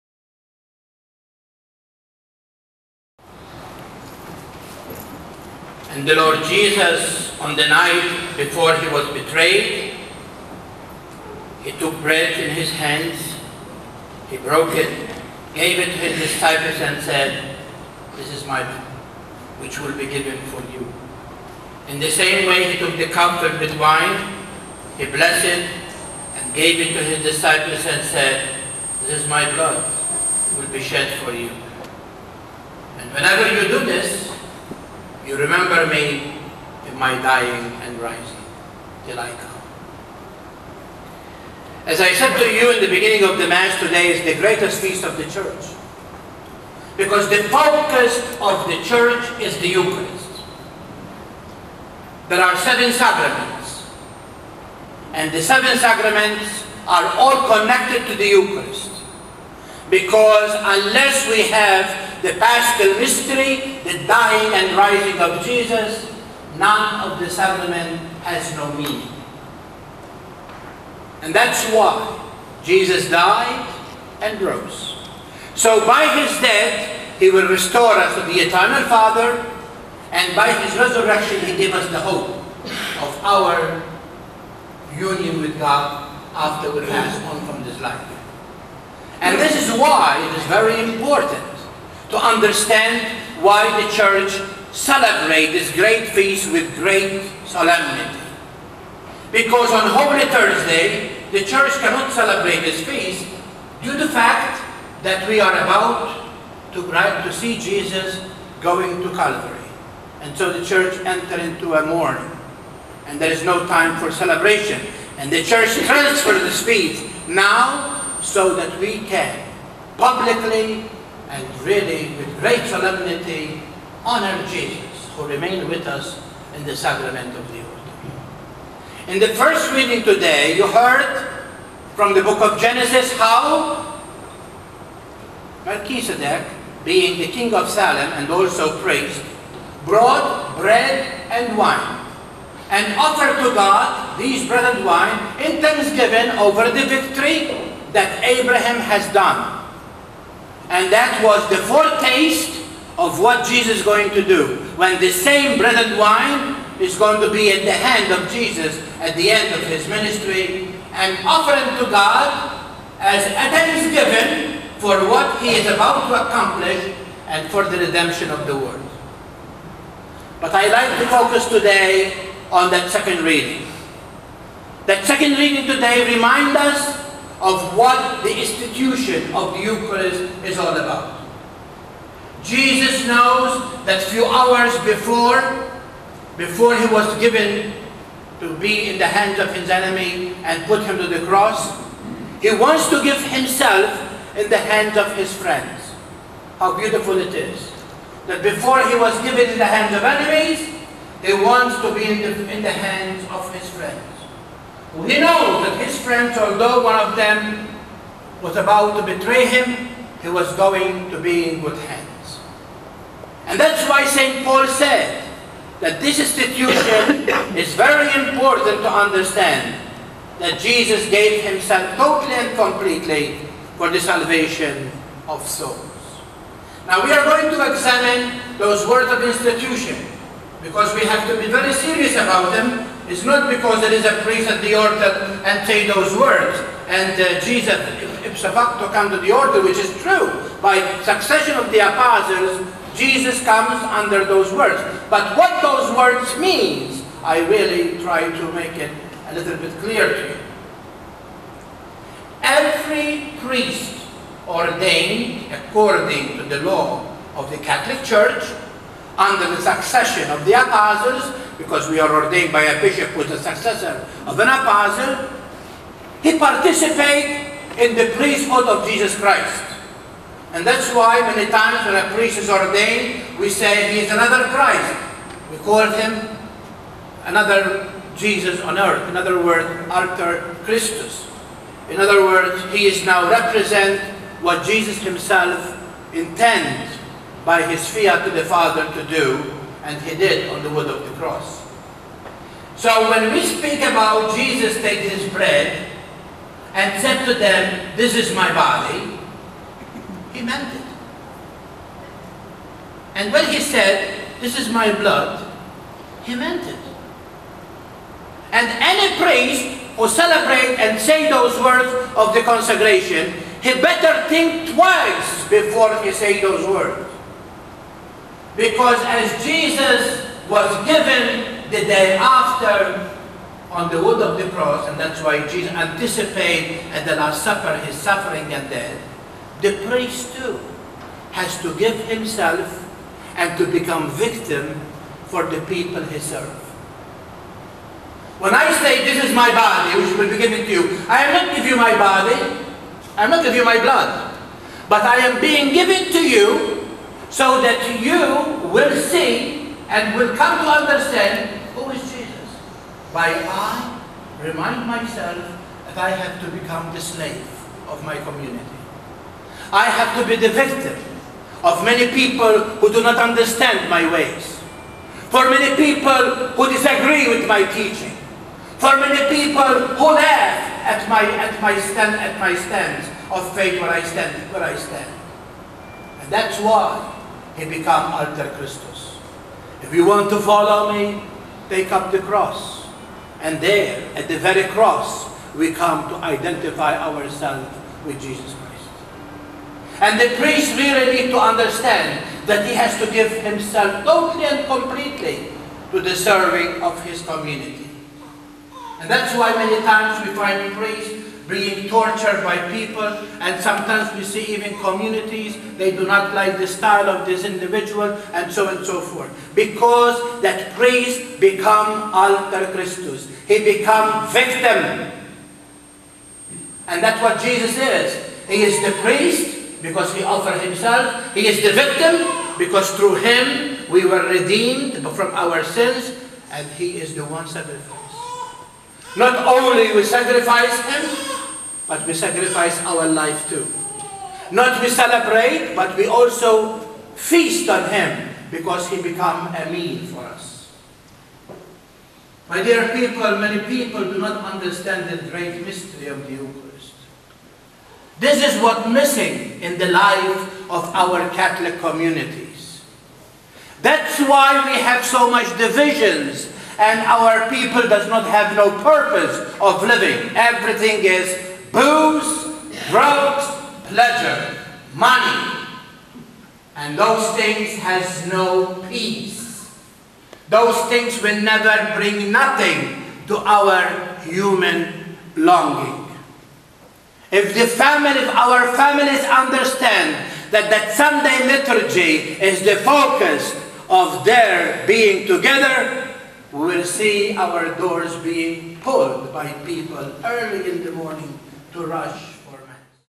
2010.6.6 Part 1 of 2 Fr. Carmel's Homily.mpg
In part 1 of 2, Fr. Carmel begins to explain to us the meaning of Corpus Christi!